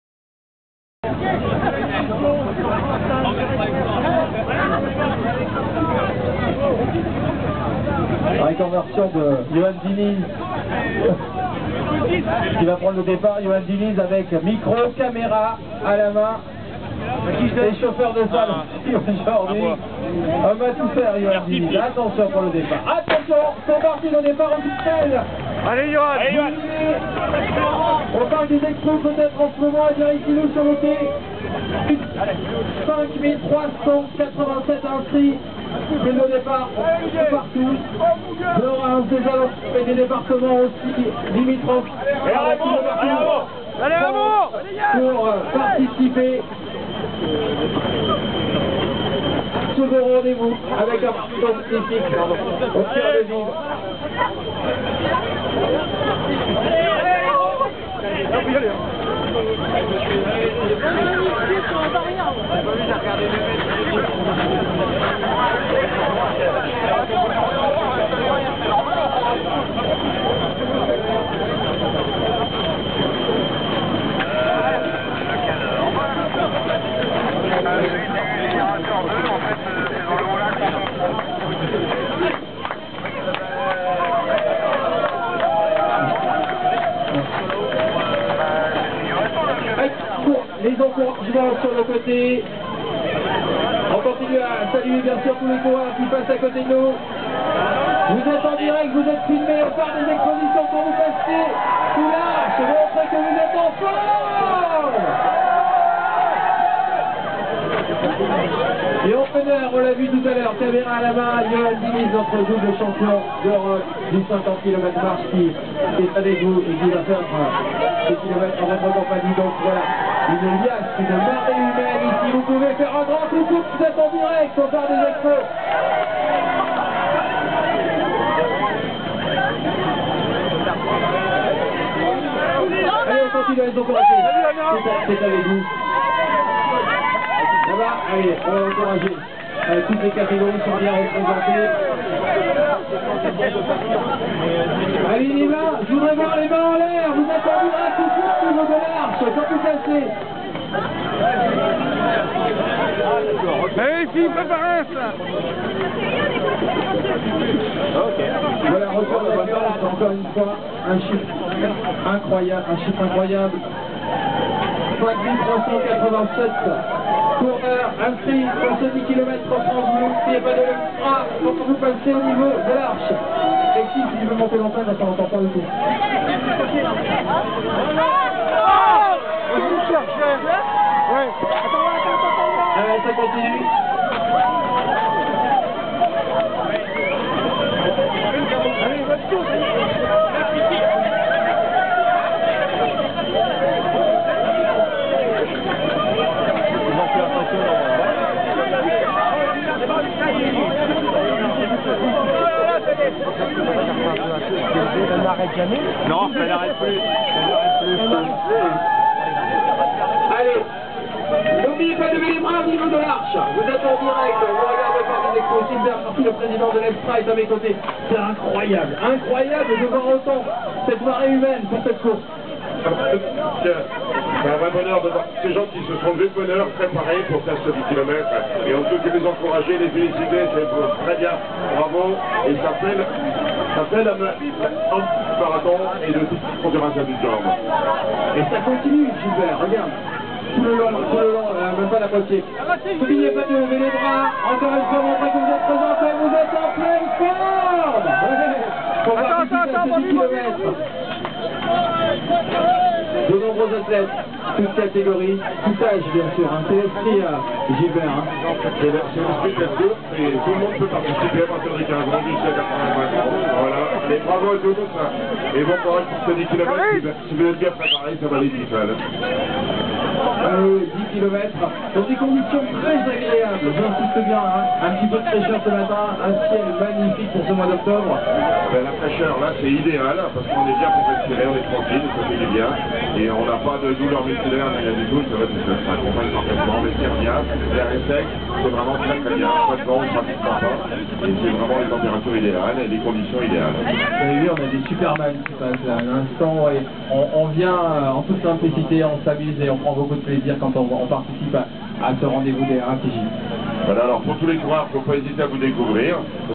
Reims RATJ 16 octobre 11 (1)
Reims
Reims A Toutes Jambes
RATJ
16 octobre 2011
Départ du 10 kilomètres